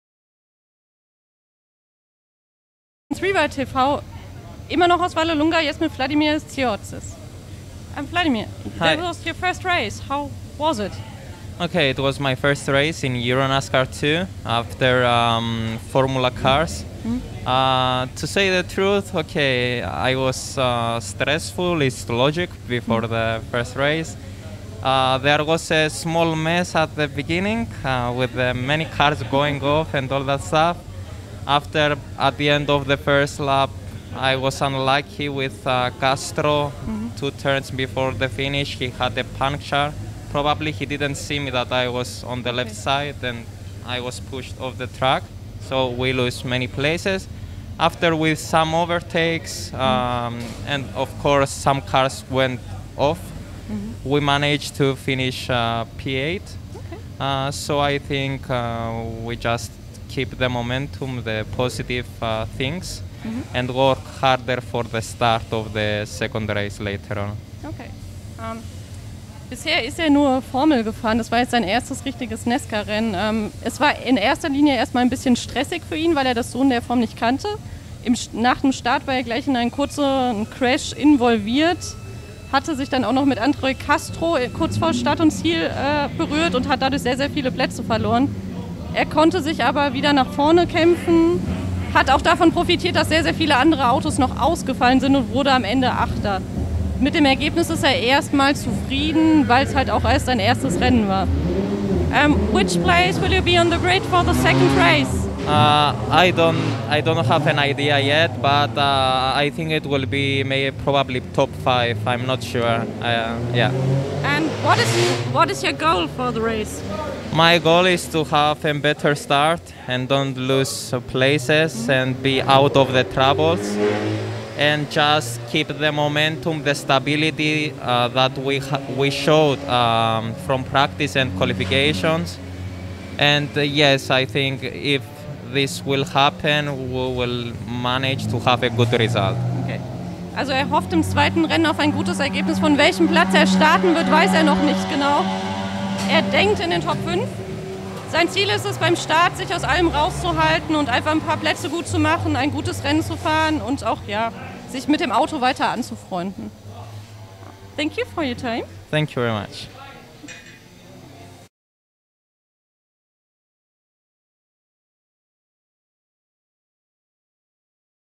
V.Tziortzis/Threewide TV Interview /Vallelunga_Round 1 of Nascar Whelen Euro Series 2020
Threewide TV interview in Vallelunga during the Round 1 of Nascar Whelen Euro Series 2020 with Vladimiros Tziortzis (EN2 driver of Alex Caffi Motorsports & Race Art Technology)